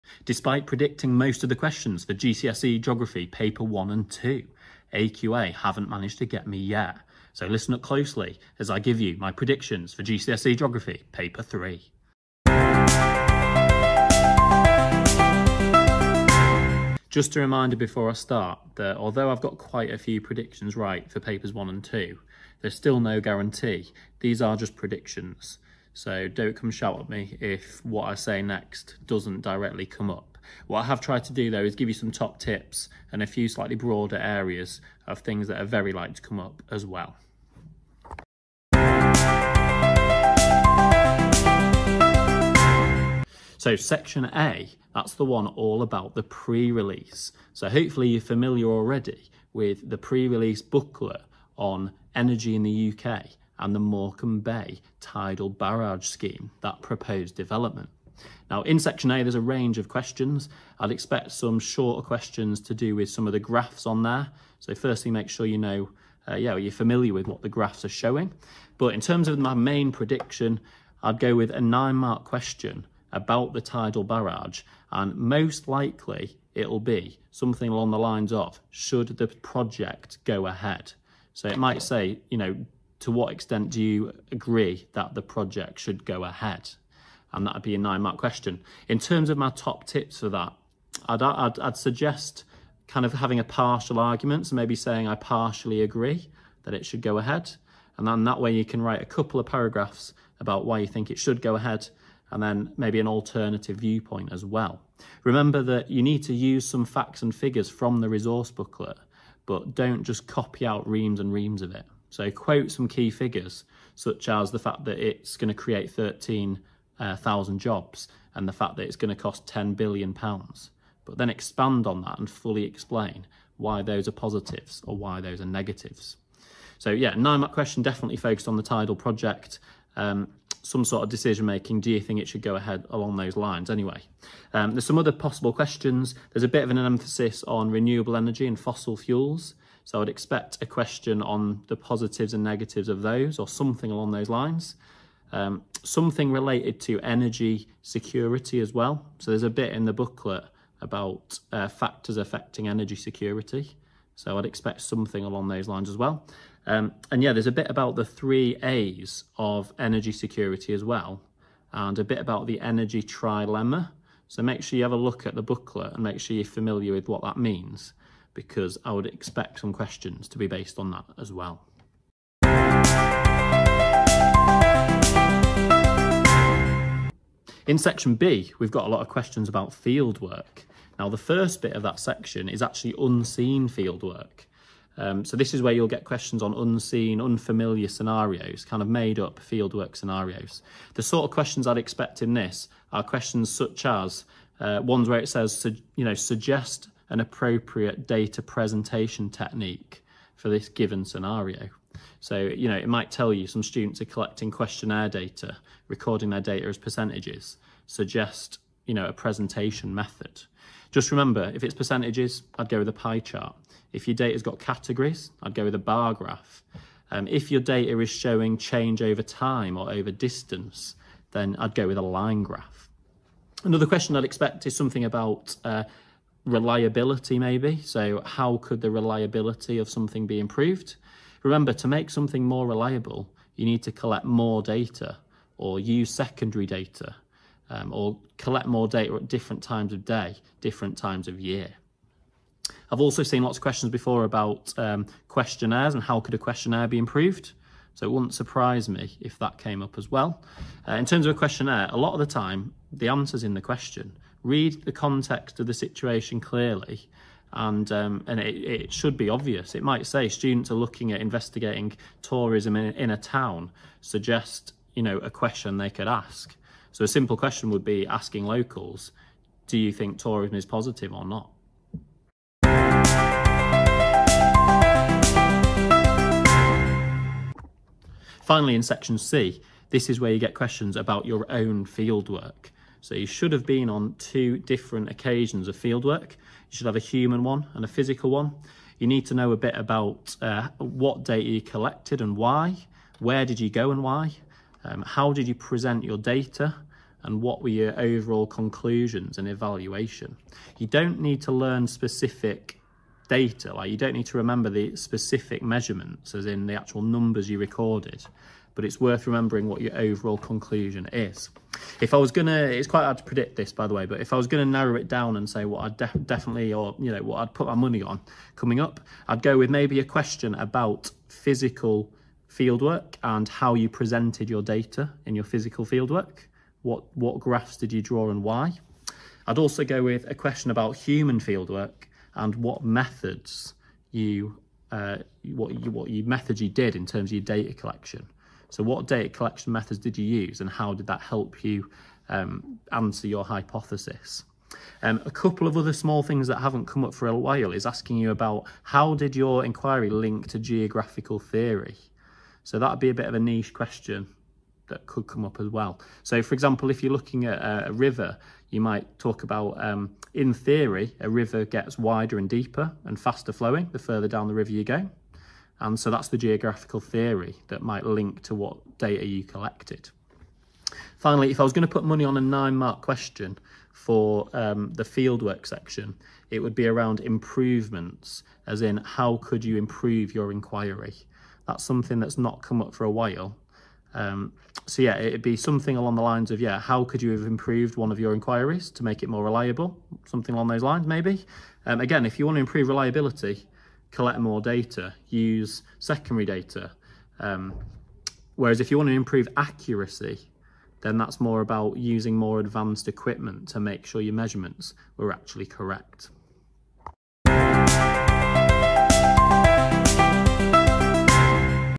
GCSE Geography Paper 3 Predictions AQA
Predicted questions/topics for AQA GCSE Geography Paper 3 2025

Issue evaluation/pre release - Morecambe Bay Tidal Barage

unseen Fieldwork

Fieldwork - Human and Physical